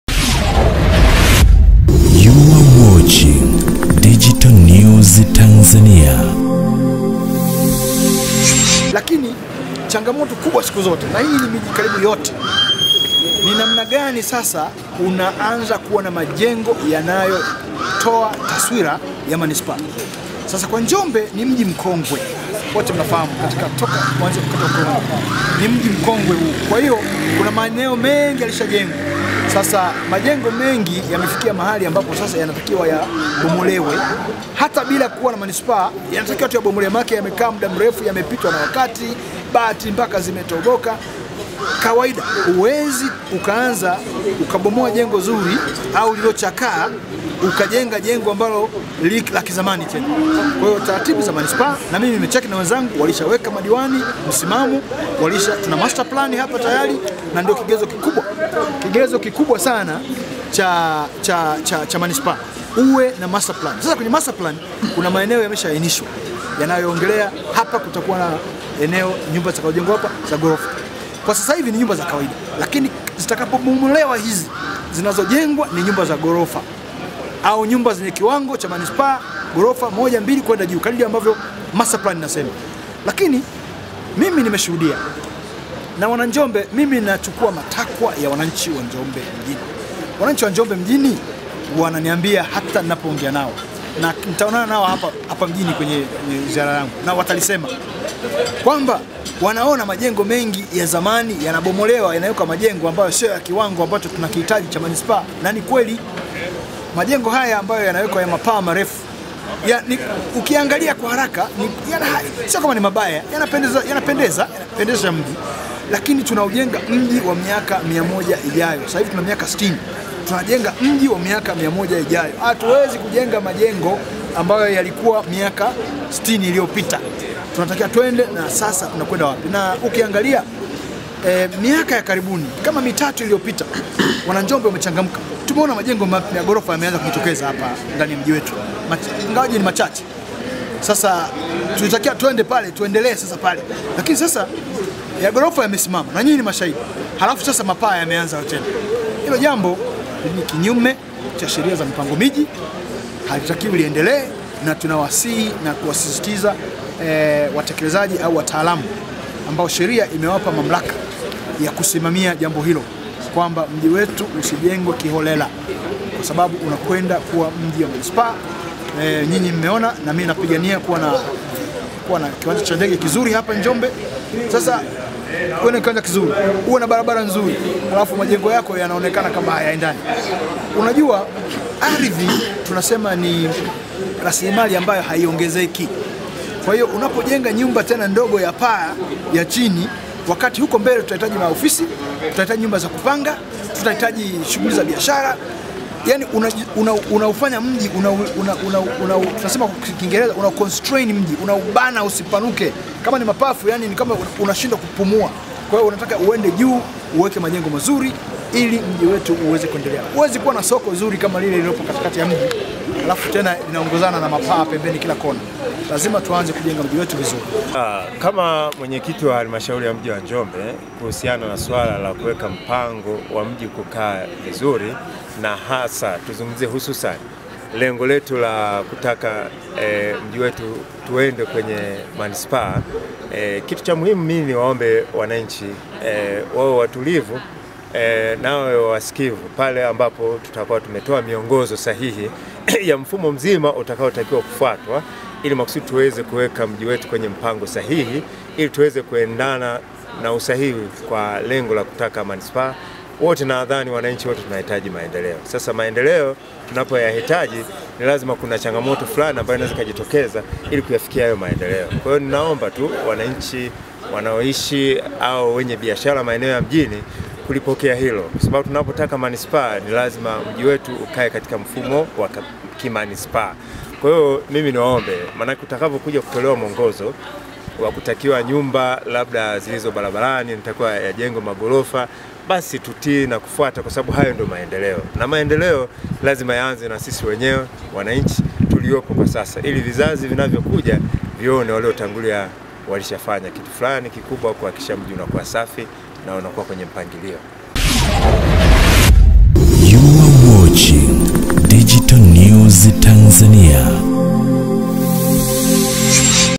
Mbunge wa Njombe Mjini Deo Mwanyika Afunguka Haya Njombe Mji Kupata Hadhi ya Manspaa/
Mbunge wa Jimbo la Njombe Mjini Deodatus Mwanyika Na Mwenyekiti wa Halmashauri ya Mji wa Njombe Erasto Mpete Wamewataka Wananchi wa YHalmashauri hiyo kuanza kubadiri mfumo wa ujenzi  wa Majumba yao ili kuupa Hadhi mji wa Njombe na Baadaye Kupewa hadhi ya Manispaa.